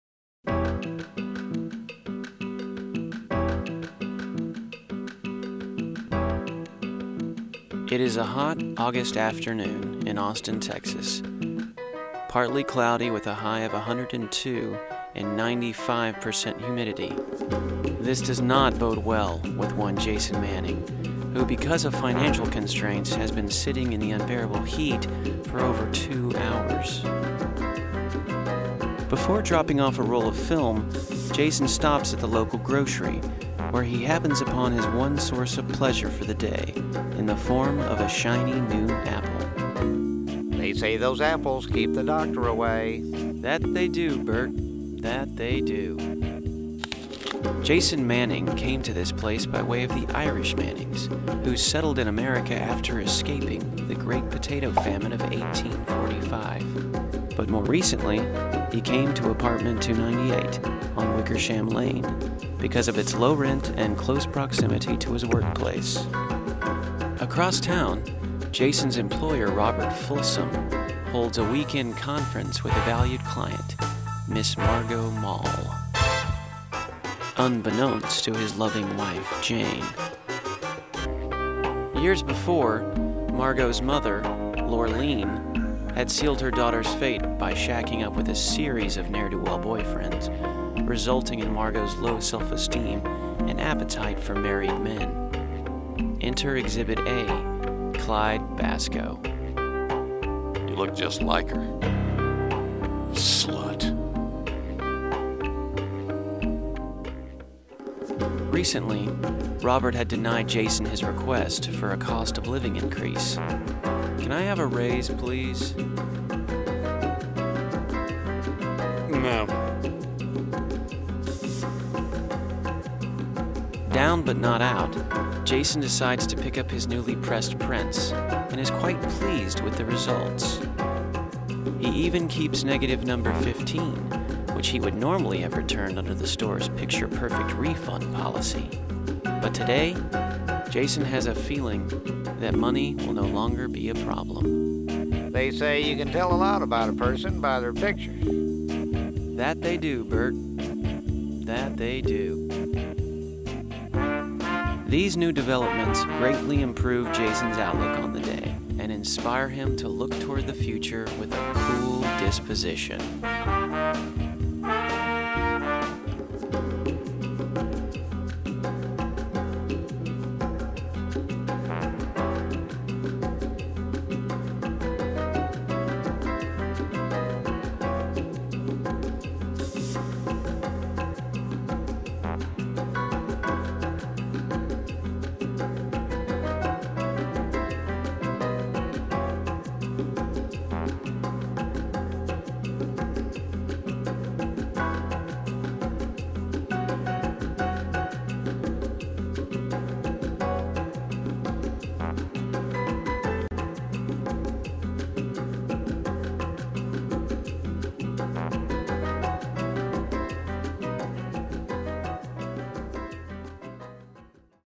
Jason and the High Cost of Electricity
Jason must turn to crime in order to afford his ever-increasing electric bill during a hot Austin summer.

Created by 05min Productions: Animations, Short Films, and Documentaries.